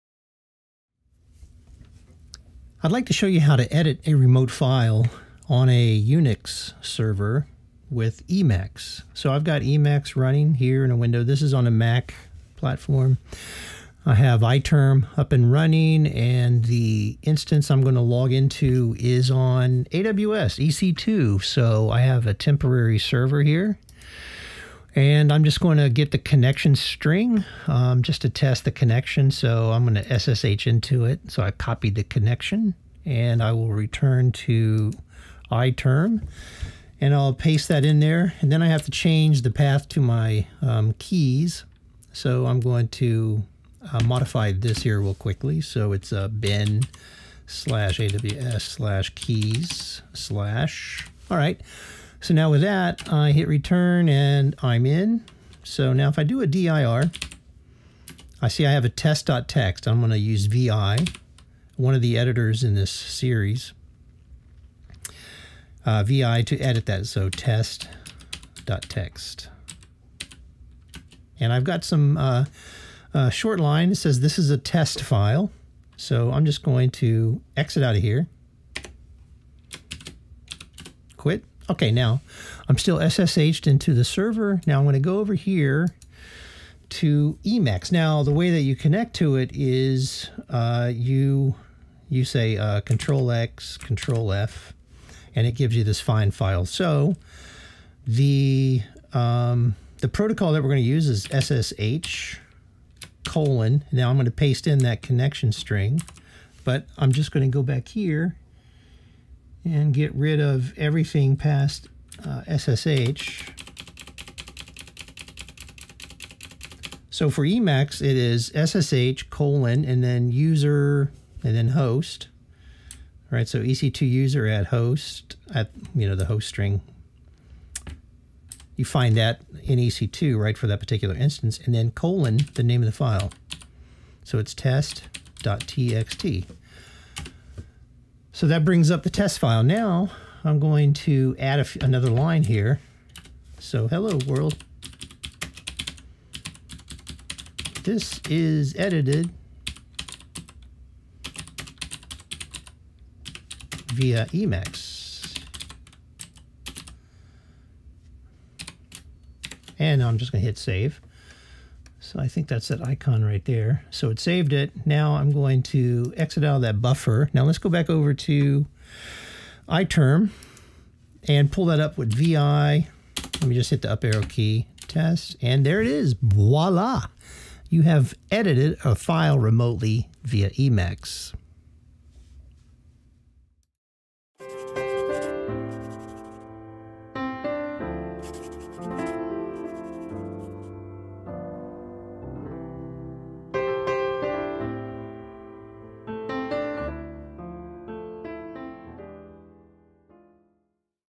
Edit Remote Files with Emacs
In this video I show you how to use Emacs to edit a file on a remote AWS EC2 Linux server